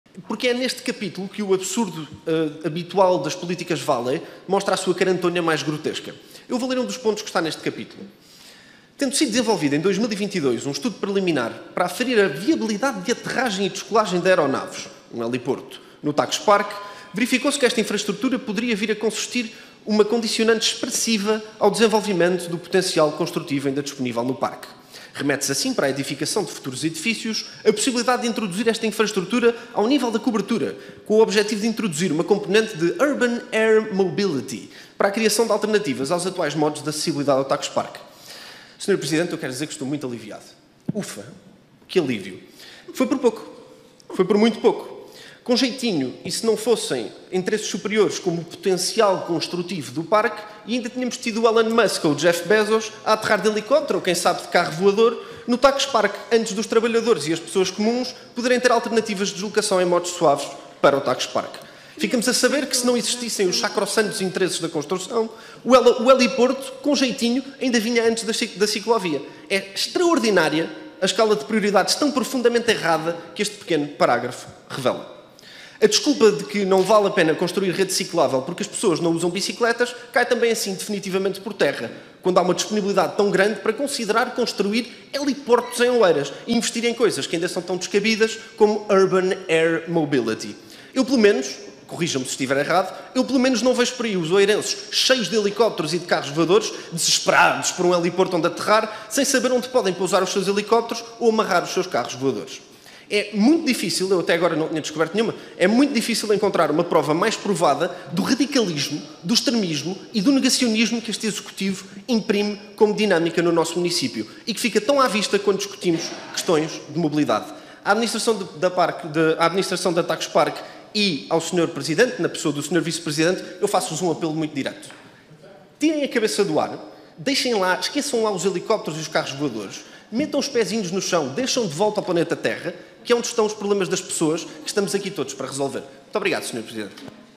Helicópteros em Oeiras?!
As políticas de Isaltino Morais resumidas numa frase: Os munícipes de Oeiras continuam a demorar uma eternidade para ir de Algés até ao TagusPark de transportes, mas a prioridade de Isaltino para a mobilidade são helicópteros.
Oeiras Valley em todo o seu esplendor.